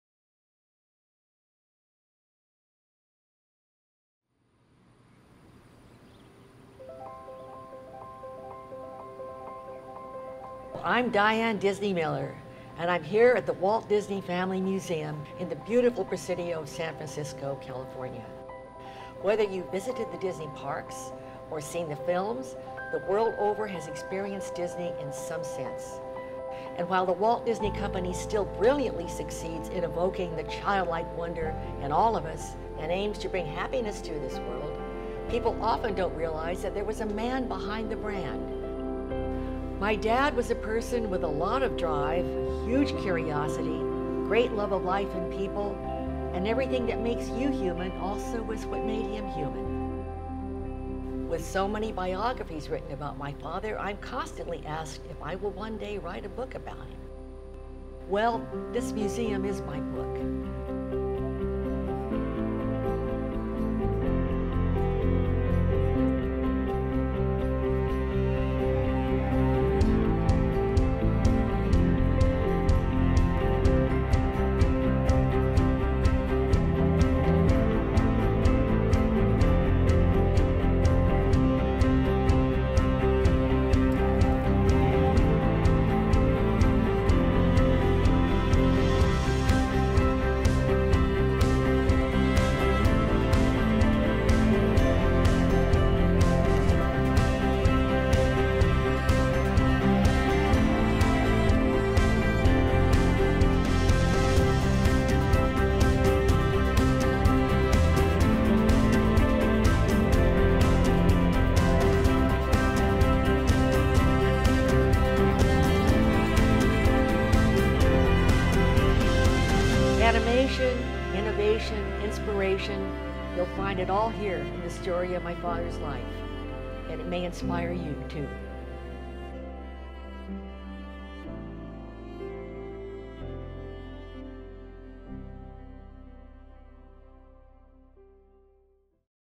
The Walt Disney Family Museum: Animating a Legacy
Explore the remarkable life story of Walt Disney—the man who raised animation to an art, tirelessly pursued innovation, and created a distinctly American legacy that transformed the entertainment world. Located in the scenic San Francisco Presidio, the museum features interactive galleries and state-of-the-art exhibits narrated in Walt's own voice. Get to know the man behind the magic through early drawings, cartoons, movies, music, listening stations, more than 200 video screens, and a spectacular model of Disneyland.